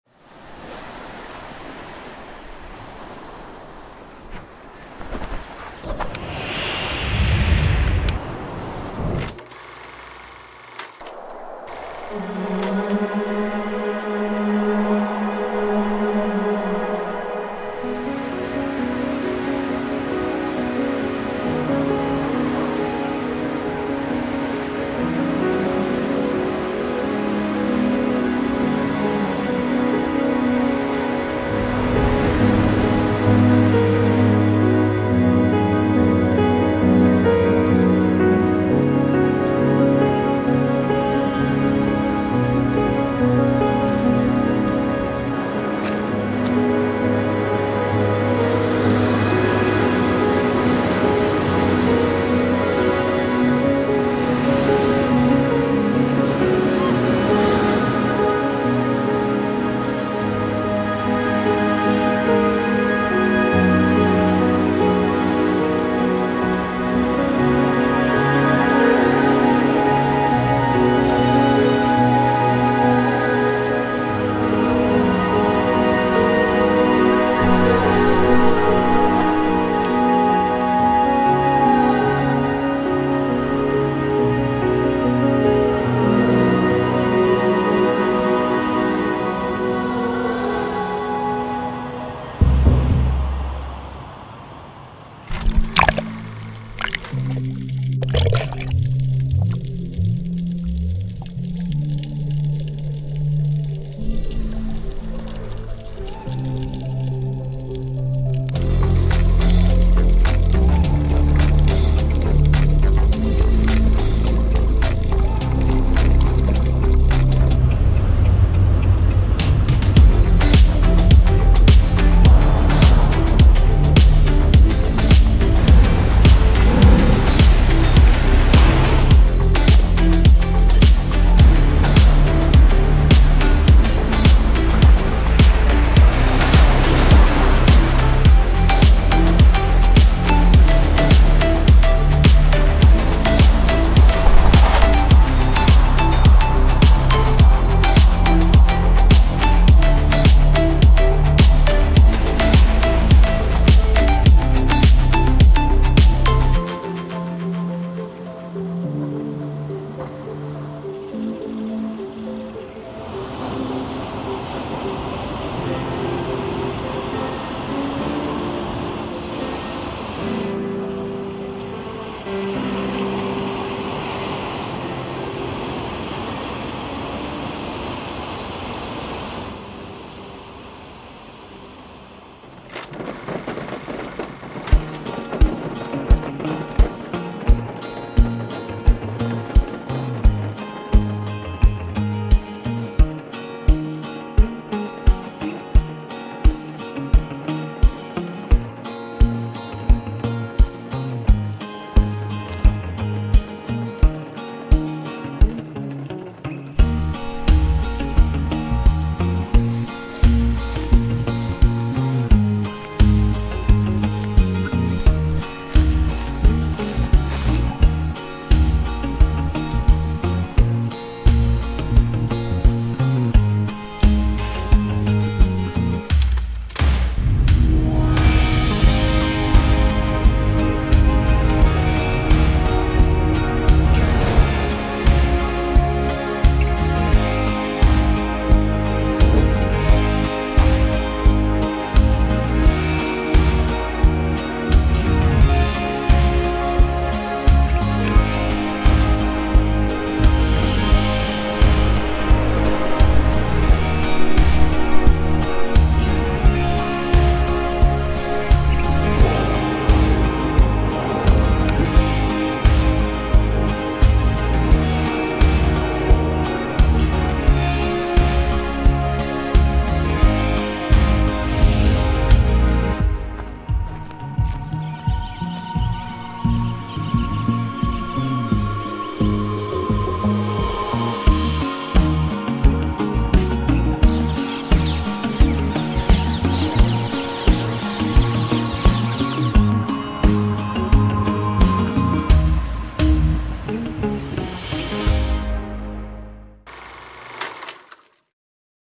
Kauai 2020 | Travel video | a6300
We were lucky enough to travel to Kauai in February 2020, just before the pandemic began.  The video captures the various places we visited during our trip.